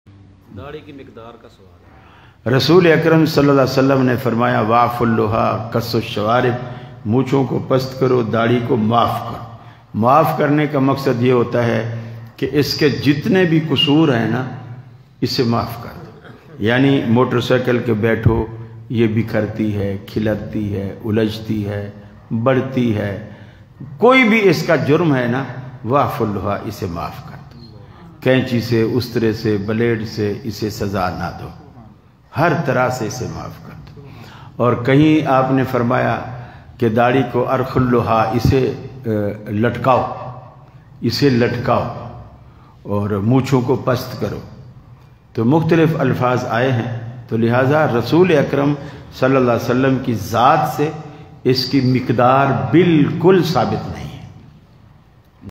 داڑھی کی مقدار کتنی ہونی چاہیے؟ | 0:56 | محترم جناب قاری خلیل الرحمان جاوید صاحب حفظہ اللہ تعالی
نشست سوال و جواب گوجرانوالہ پاکستان